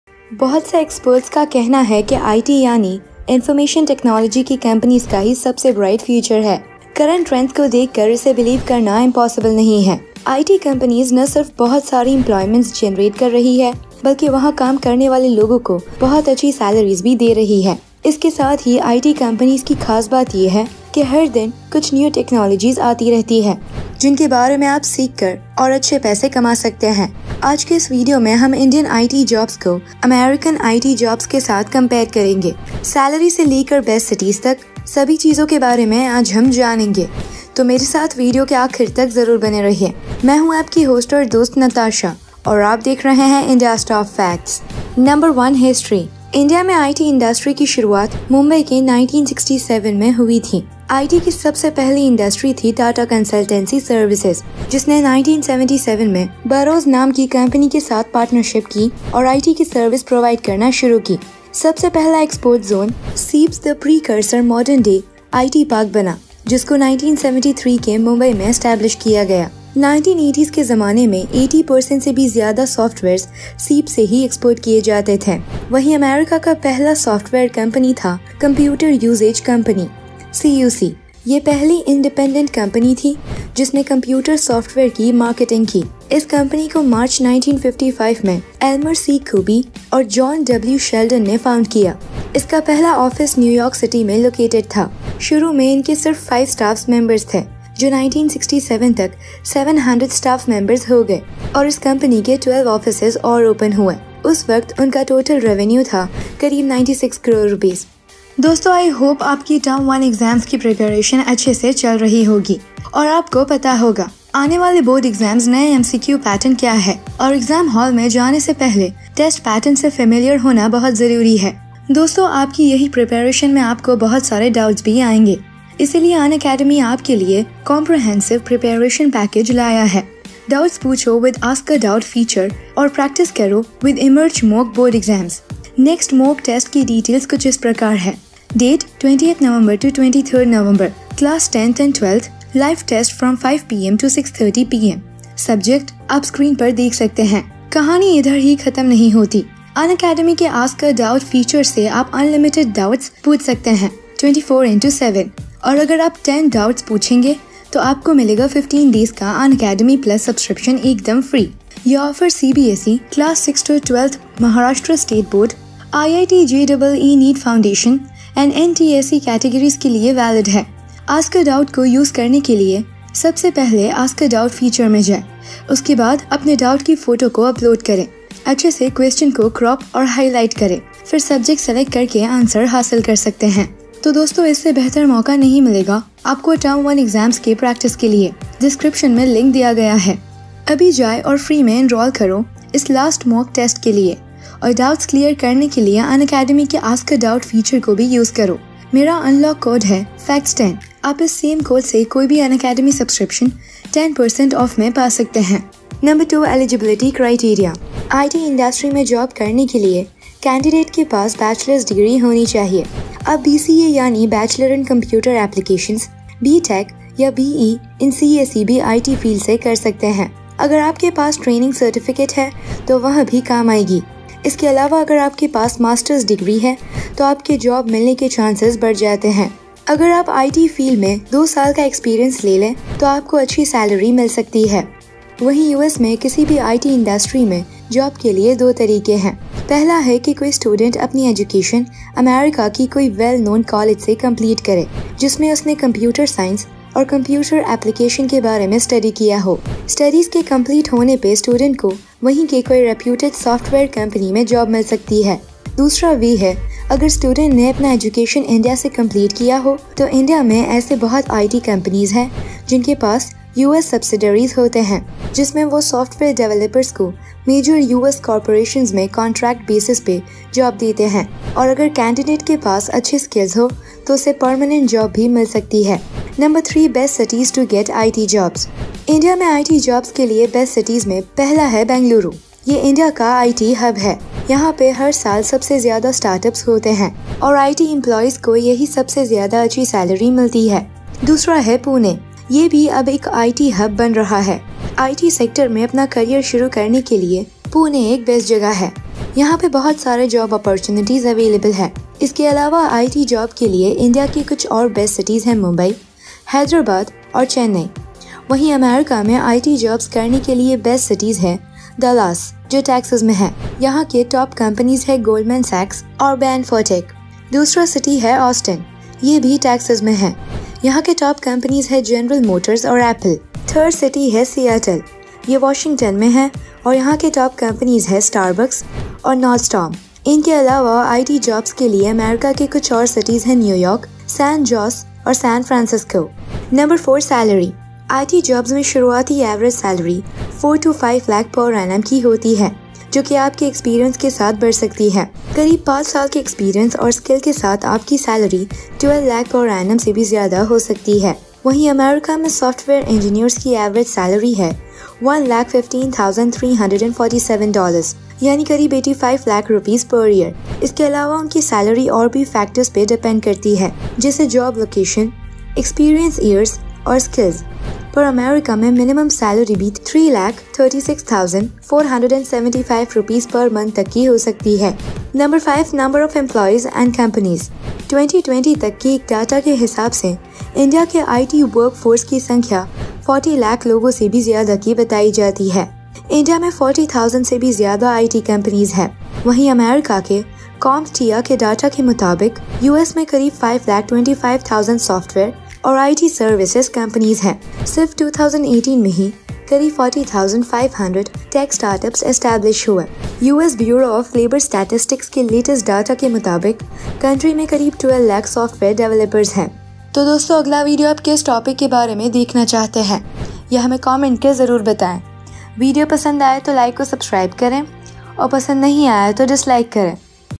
IT Jobs in India vs IT Jobs in USA Full Comparison UNBIASED in Hindi
Ask 10 Doubts, Win 15 days Unacademy Subscription for FREE,

IT Jobs in India vs IT Jobs in USA
it jobs salary in india
it jobs salary in USA